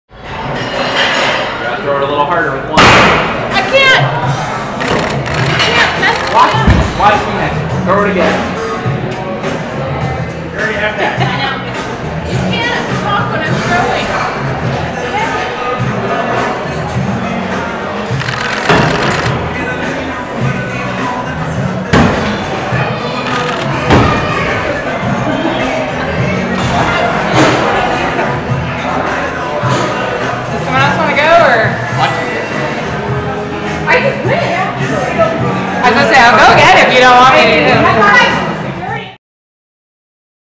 Ax throwing at Vancouver's Hopworks brewery
Ax throwing is now legal at bars in Washington. But don't expect to hold a beer and an ax in a lane. The lanes at Vancouver' Hopworks Urban Brewery are managed by Celtic Ax Throwers — and are hopping with people ready to try a hand at tossing an ax.